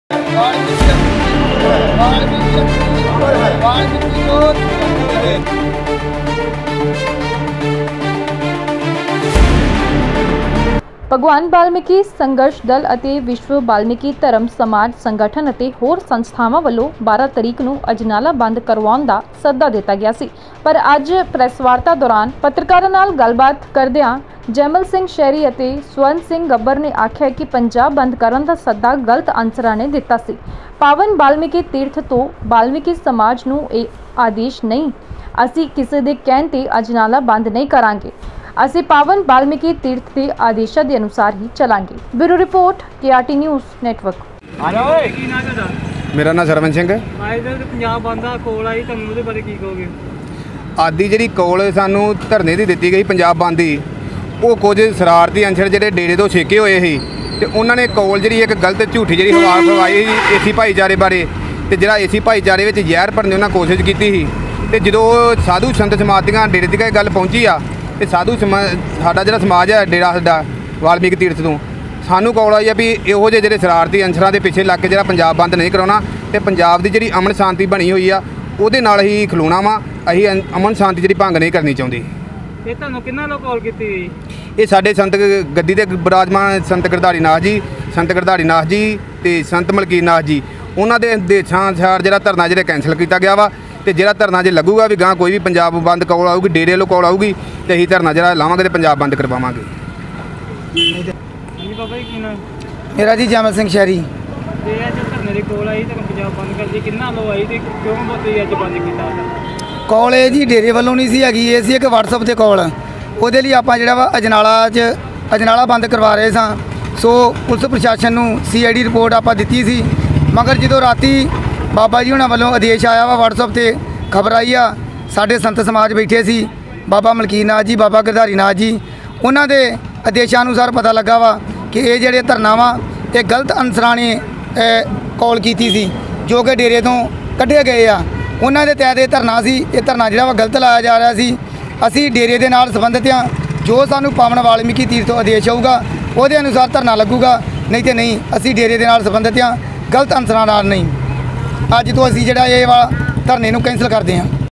Amritsar : आखिर बंद की कॉल होने के बावजूद भी क्यों नहीं हुआ आज अजनाला बंद, जानिए इस रिपोर्ट में
Amritsar : आखिर बंद की कॉल होने के बावजूद भी क्यों नहीं हुआ आज अजनाला बंद, जानिए इस रिपोर्ट में // Live